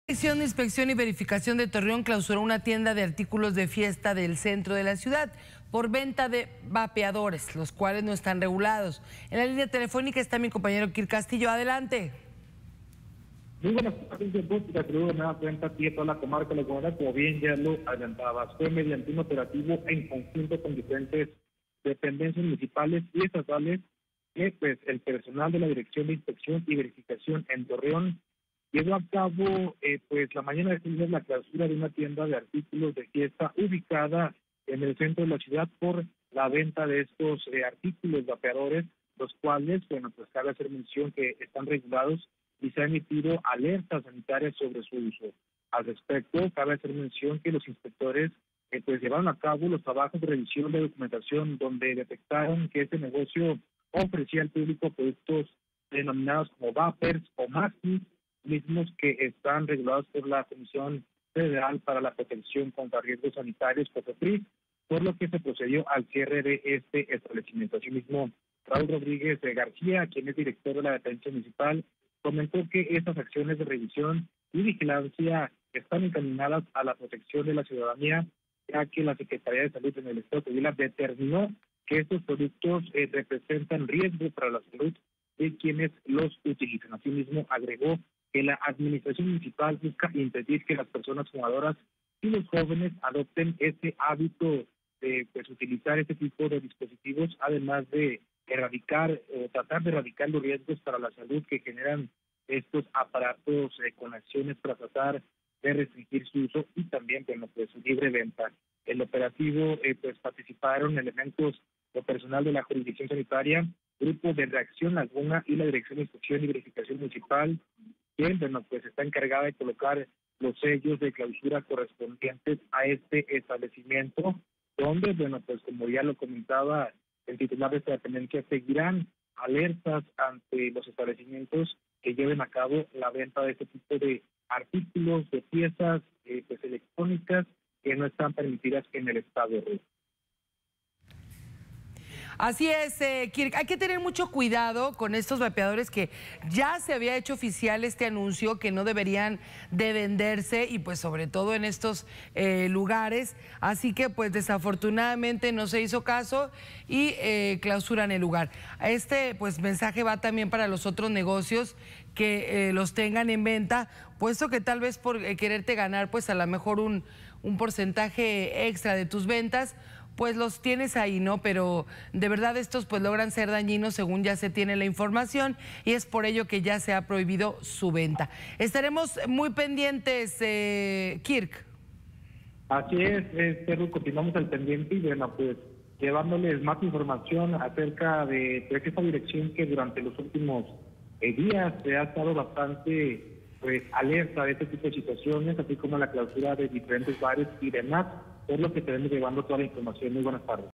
Clausuran tienda por vender 'vapes'
Un negocio dedicado a la venta de artículos para fiesta fue clausurado en la Laguna pues ofertaba cigarros electrónicos (vapes) no regulados.